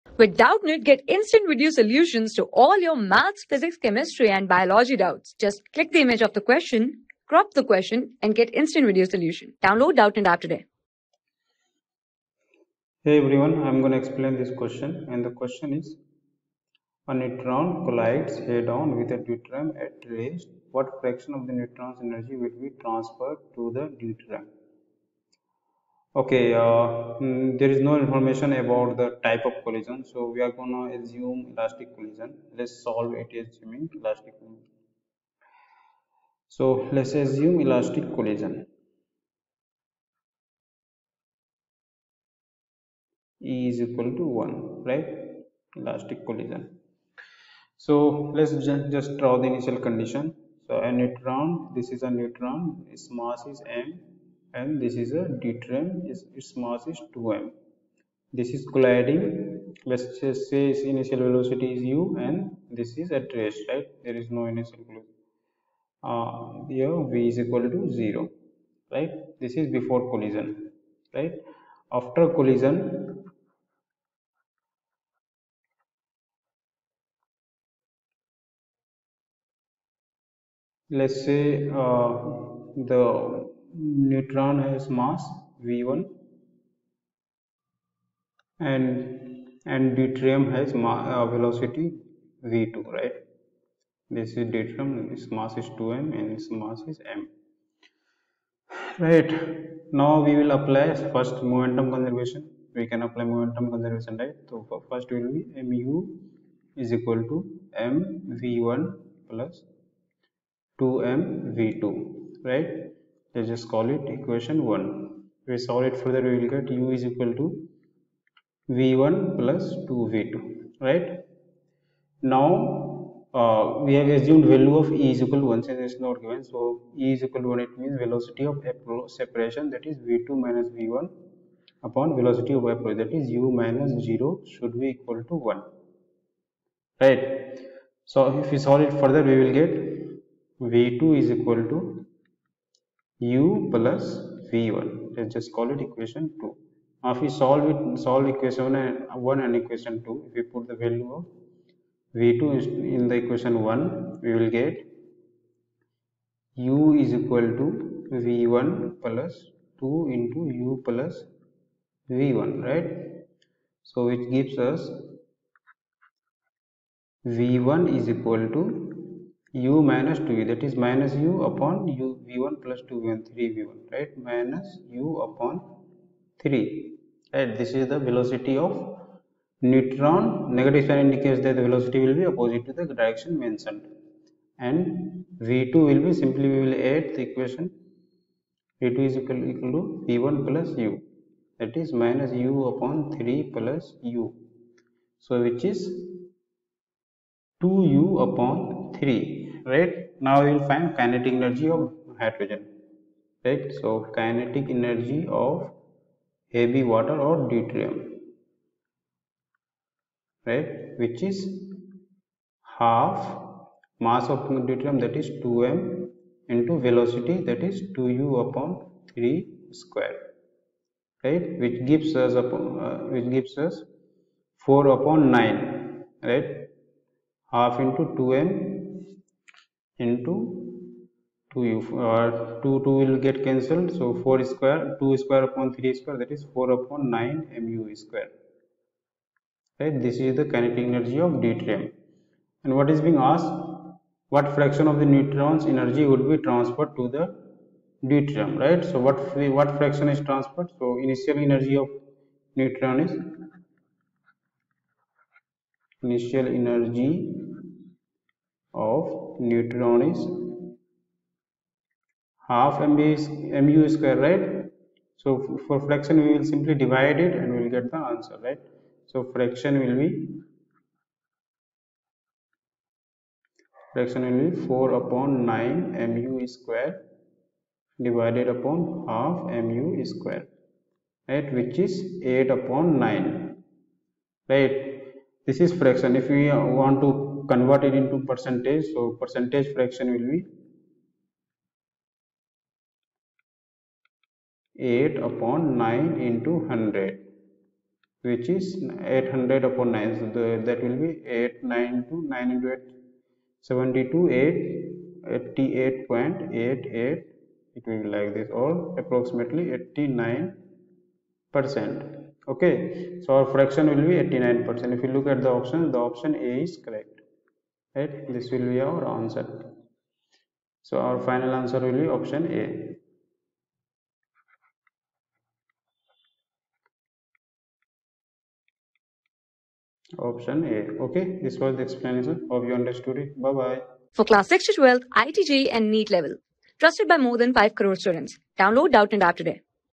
A neutron collides, head-on with a deuterium at rest. What fraction of the neutron\'s energy wou...
A neutron collides, head-on with a deuterium at rest. What fraction of the neutron\'s energy would be transferred to the deuterium?
Class: 11
Subject: PHYSICS
Chapter: WORK , ENERGY AND POWER
Board:NEET

👉 Have Any Query? Ask Us.